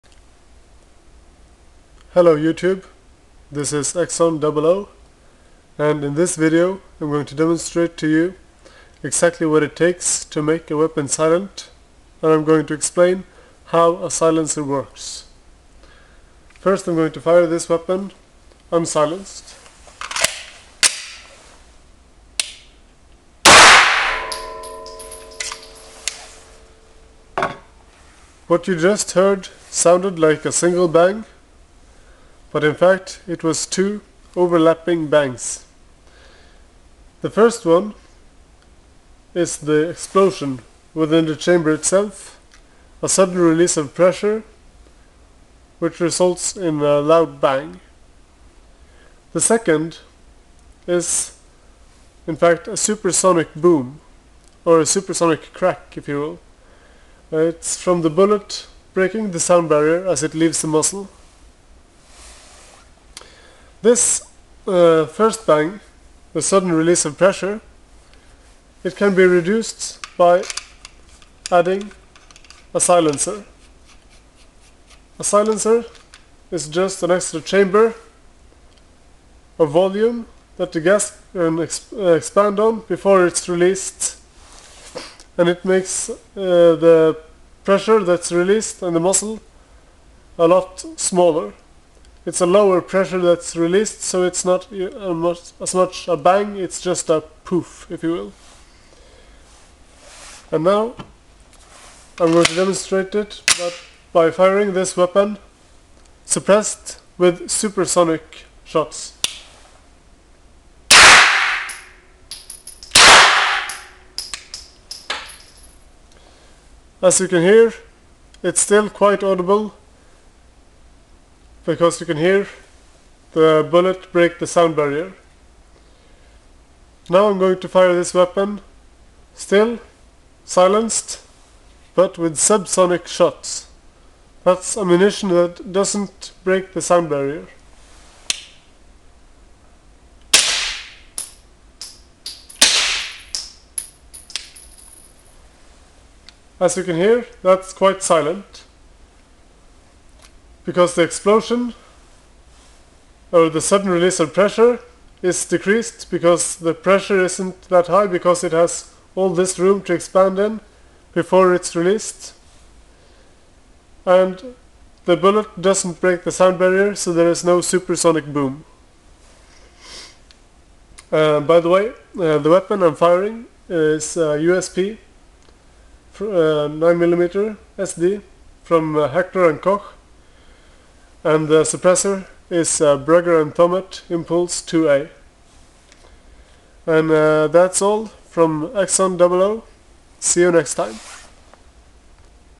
how a silencer works (Silenced HK USP 9SD)
How a silencer works:

A silencer is just an can with several baffles(although the B+T Impulse IIA has a built-in recoil enhancer to ensure reliable functioning for browning action pistols). The baffles are there to progressively slow down and deflect the expanding gases. So that when it reaches the end of the silencer its no longer a single shock wave, but a low-pressure flow.

The gun is a HK USP 9SD date code AI.
It has match trigger with trigger job.